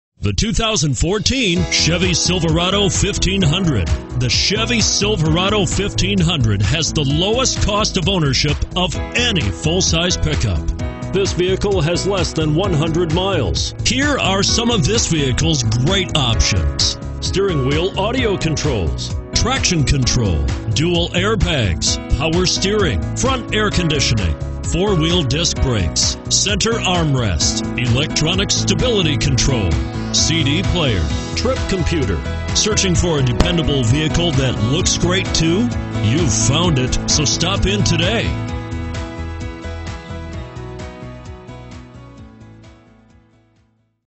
2014 Chevrolet Silverado 1500 Algona IA 4881A
2014 Chevrolet Silverado 1500 LT

6-Speed Automatic Electronic with Overdrive and 4WD. All the right ingredients! Extended Cab! This 2014 Silverado 1500 is for Chevrolet nuts looking far and wide for that perfect truck.